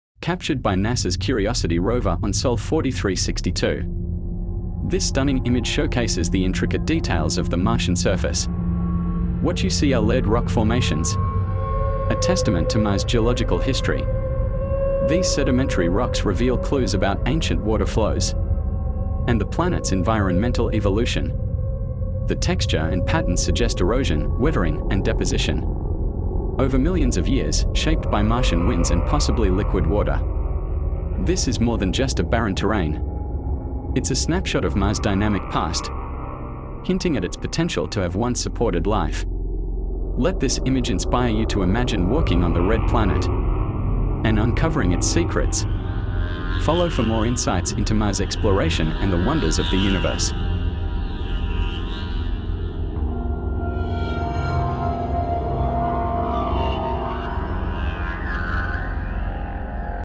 Mars view Perseverance
"Captured by NASA's Curiosity rover on sol 4362,
this stunning image showcases the intricate details of the Martian surface.

What you see are layered rock formations,
a testament to Mars' geological history.

These sedimentary rocks reveal clues about ancient water flows
and the planet's environmental evolution.

The texture and patterns suggest erosion, weathering, and deposition
over millions of years, shaped by Martian winds and possibly liquid water.

This is more than just a barren terrain;
it's a snapshot of Mars' dynamic past,
hinting at its potential to have once supported life.

Let this image inspire you to imagine walking on the Red Planet
and uncovering its secrets.

Follow for more insights into Mars exploration and the wonders of the universe.